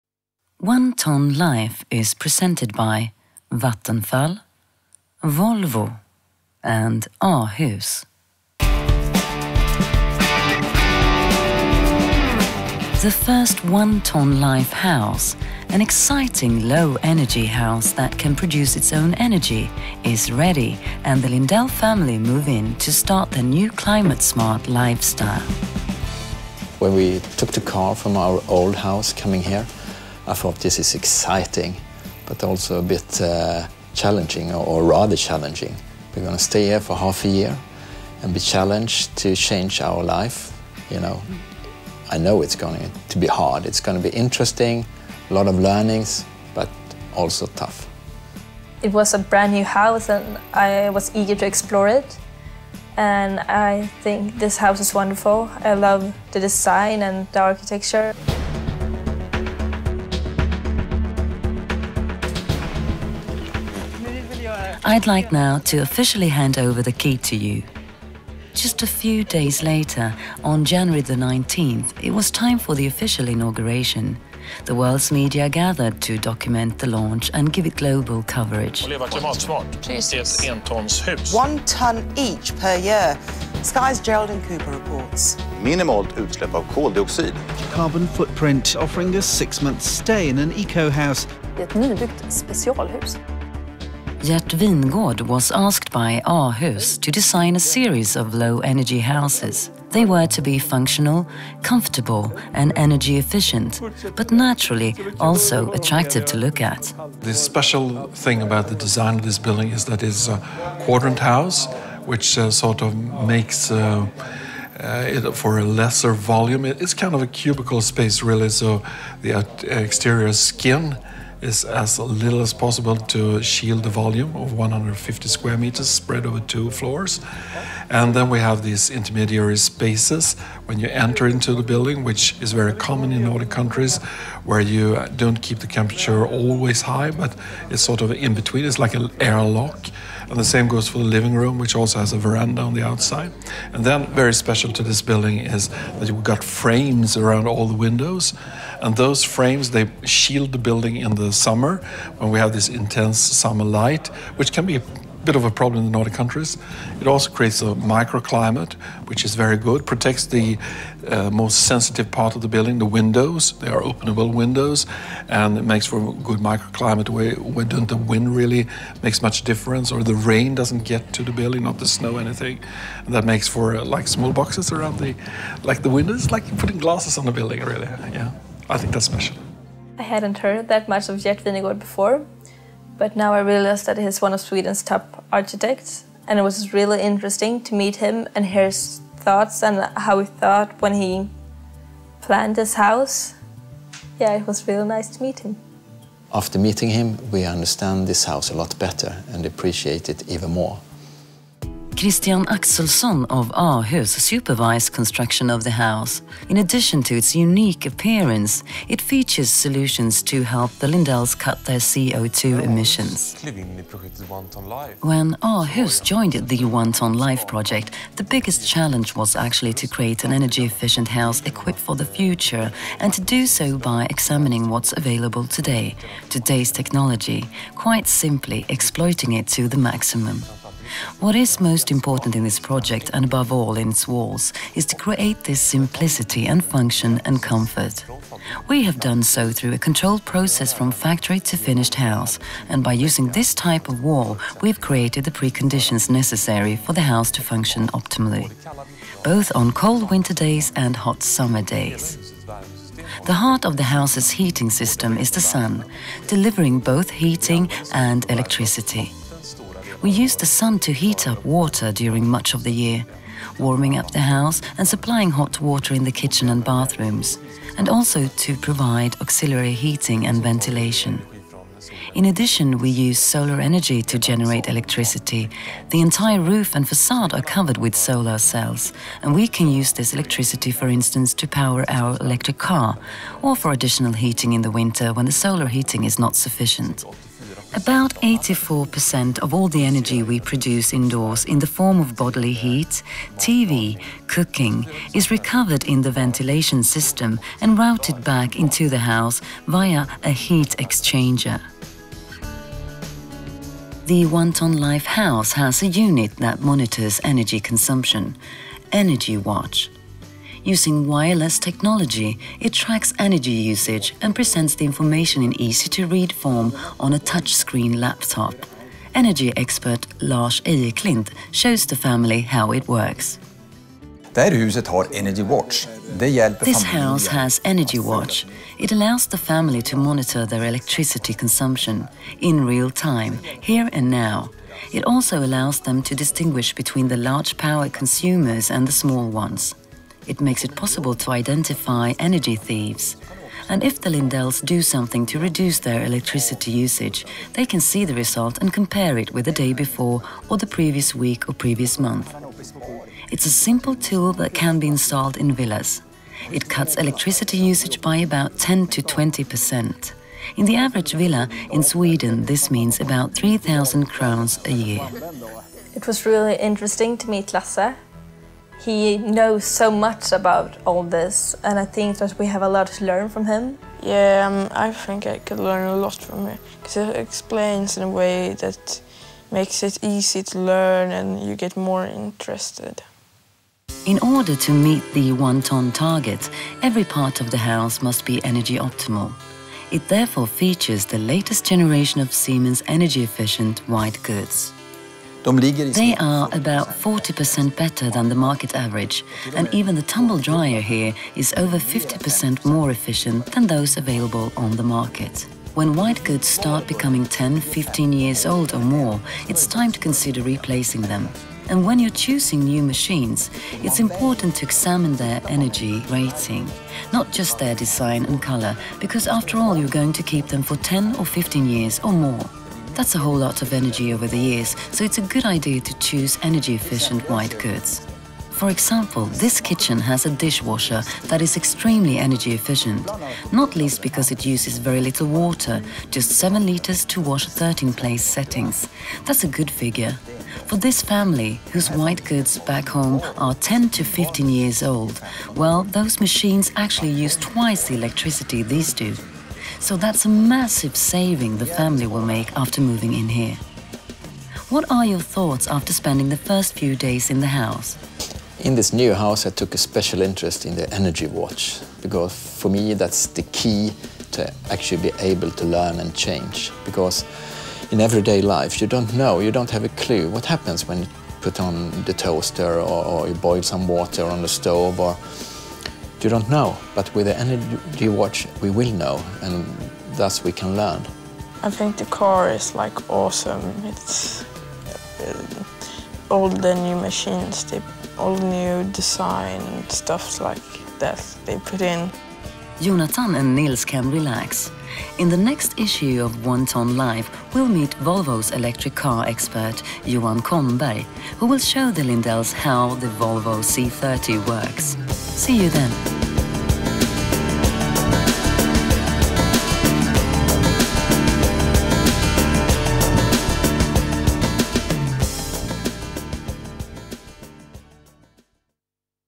Webisode #2 "Moving In" One Tonne Life in ENGLISH
The launch of One Tonne Life was fantastic. Meet the family Lindells, the house designer Gert Wingårdh, Christian Axelsson and Lars Ejeklint, and get to know more about the hidden technical gagdets thats helping the Lindells  to reach their goal of One Tonne carbon dioxide per person/year. Sky News, BBC, TV4 and ABC covered the day, along with traditional press and blogs.